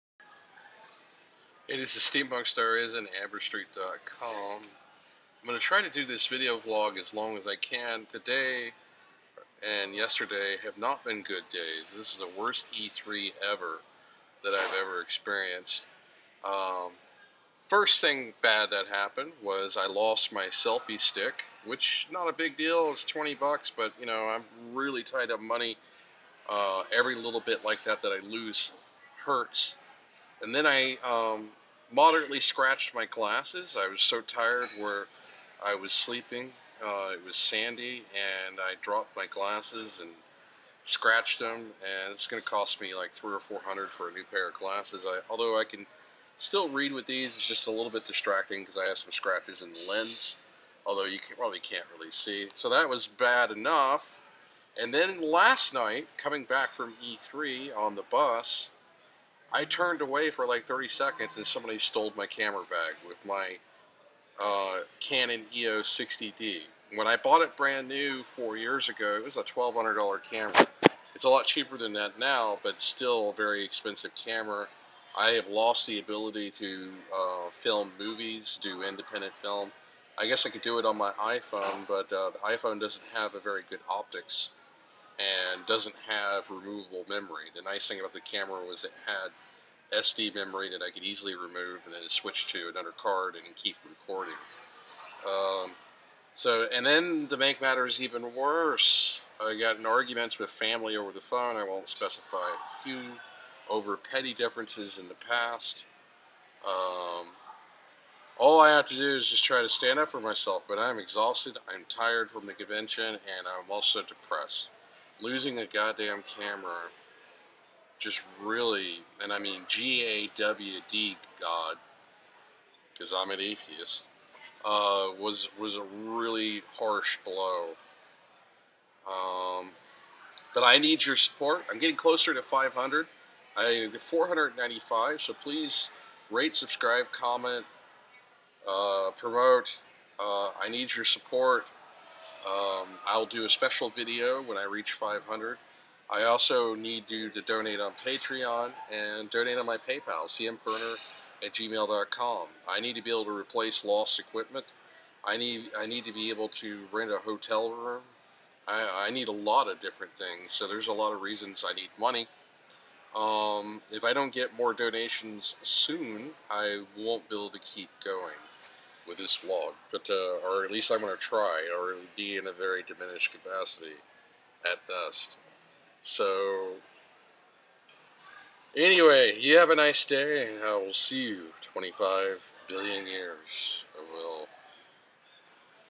Brief Update 6.19.2015
This is a brief update for June 19th 2015. Someone on the bus stole my camera and I scratched my classes. I need your support.